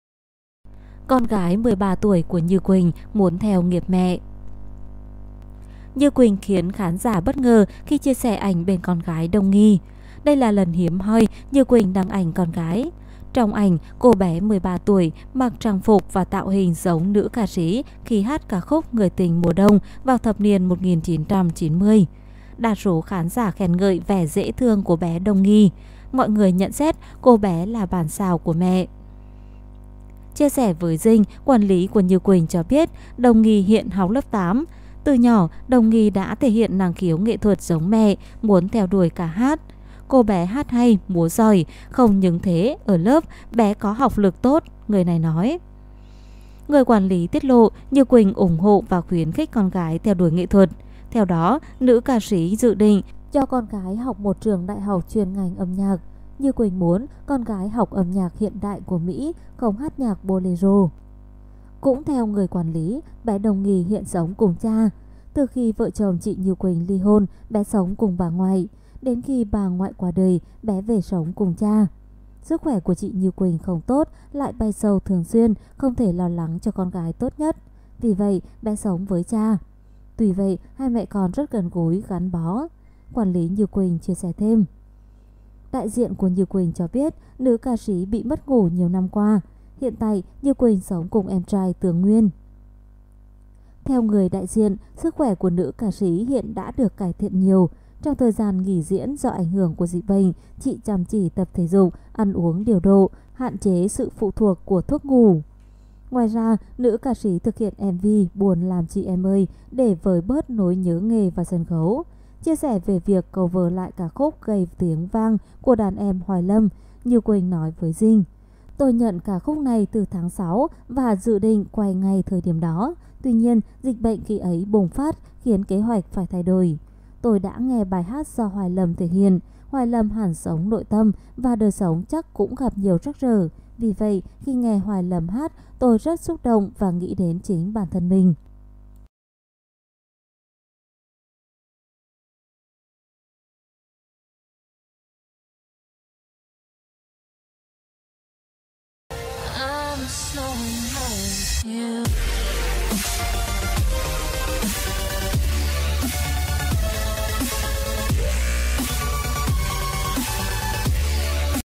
Như Quỳnh Tiết Lộ Về Con Gái Ruột Của Mình Khiến Người Hâm Mộ Vui Mừng
là Kênh tổng hợp các tin tức, sự kiện nổi bật trên tất cả các lĩnh vực trong ngày. Cập nhật nhanh và liên tục.